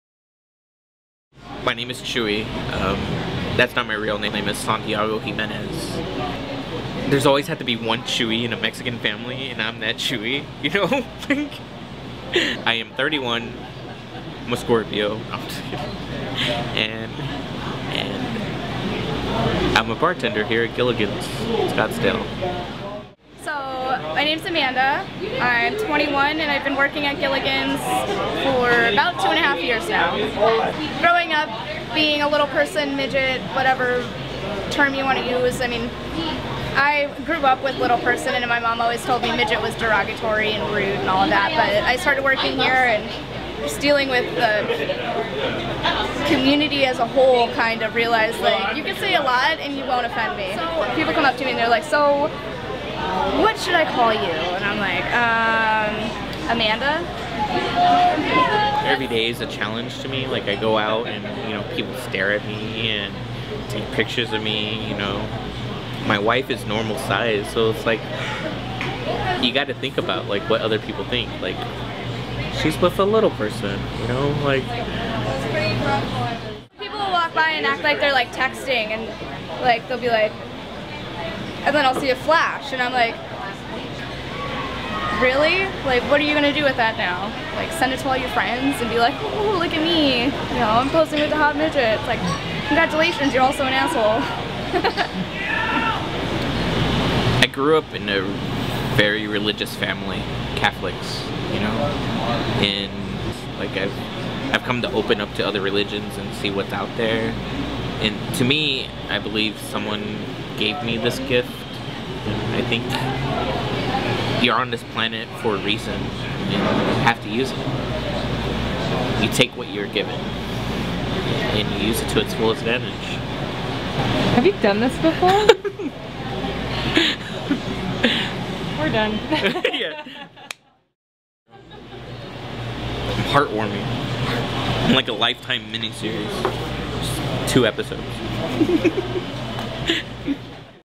Giligin's
Mary Beth Koeth travels to Arizona to interview Chuey and Amanda at Giligin's Bar.